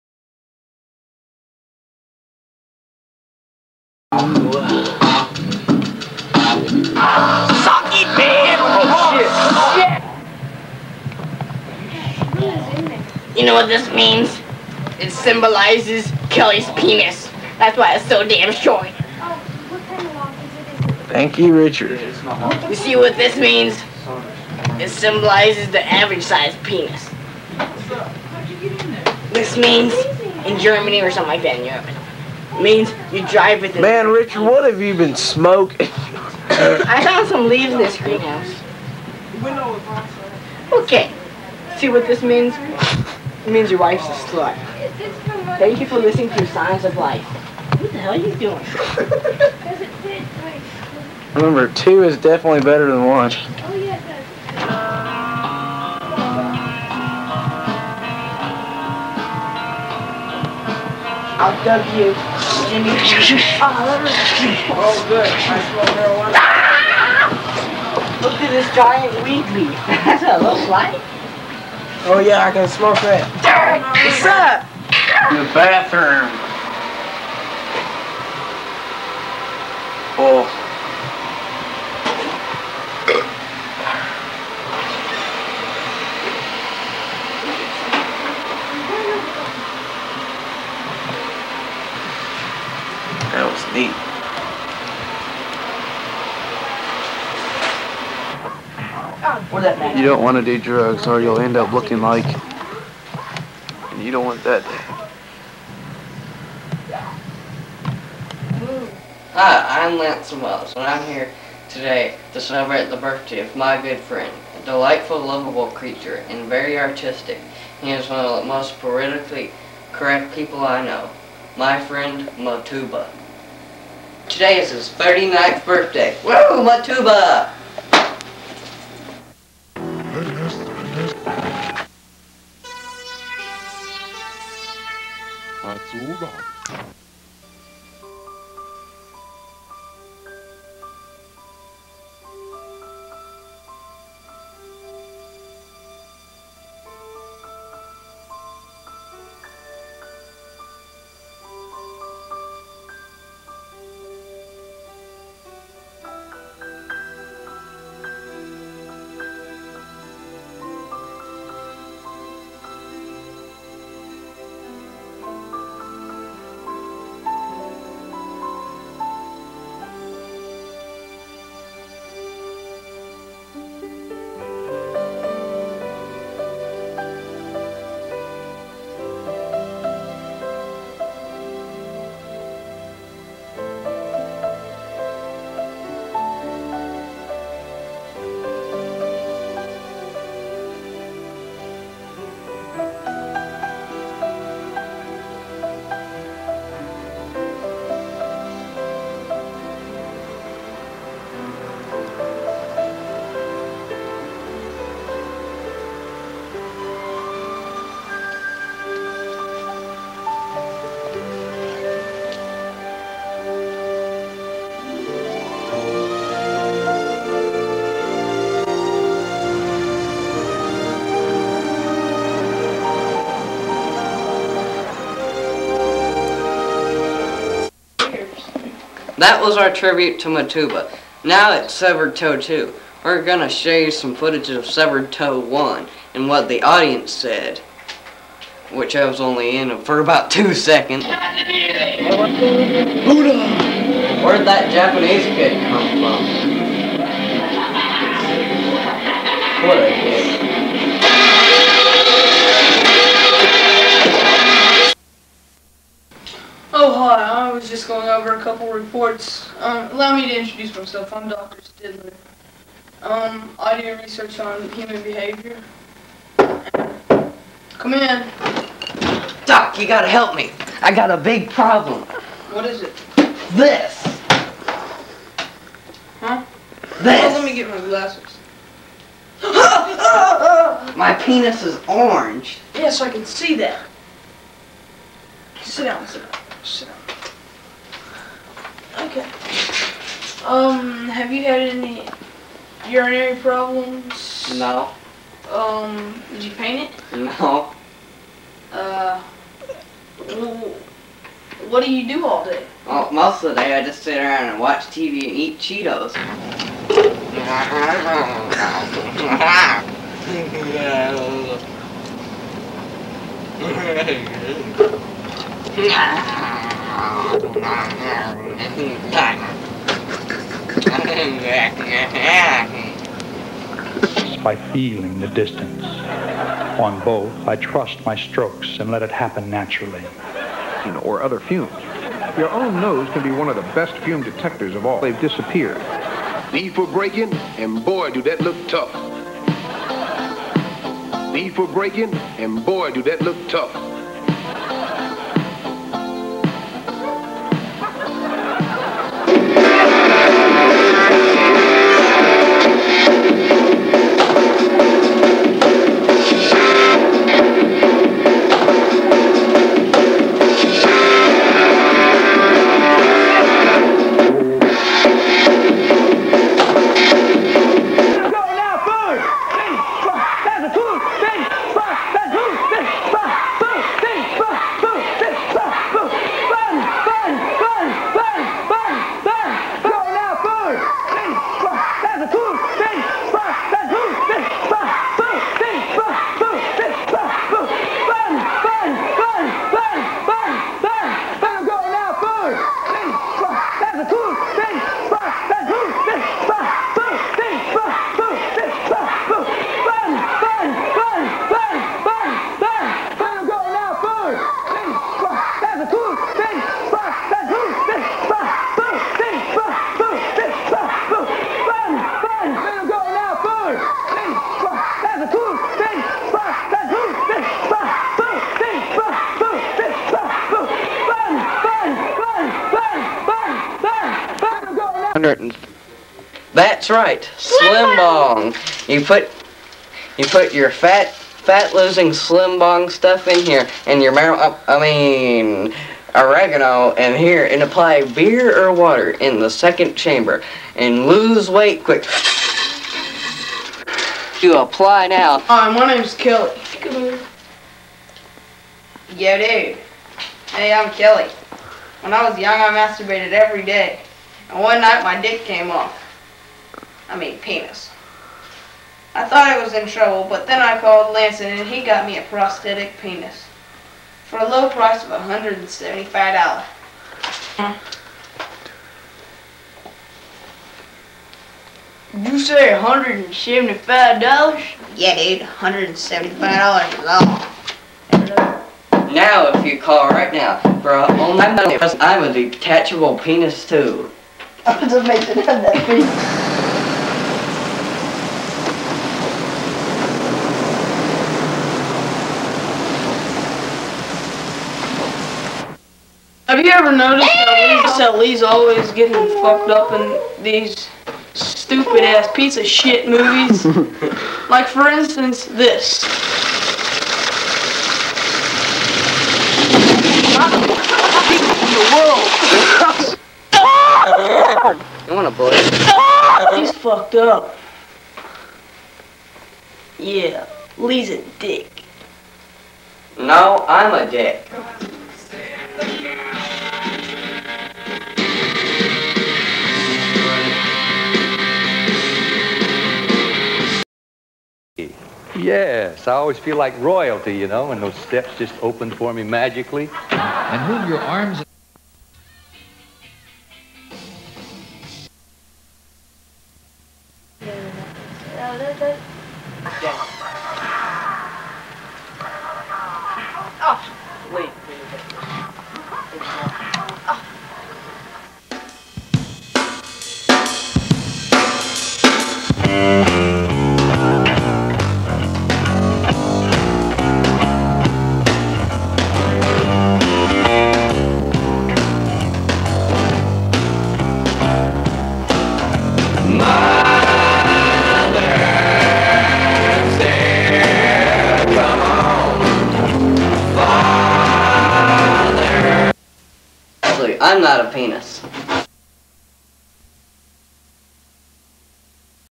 Severed Toe Pieces
circa 1995 .Taken from the end of Severed Toe 2 . Daniel Bowling, Hunter Armstrong, Richard Chen, Lee Lyons, Mason Williams, Jason Baker, Danny Bowling, Lanson Wells, Josh Saunder, Jason Baker, Josh Sullivan.
B for "Breaking!" and boy do dat look tough.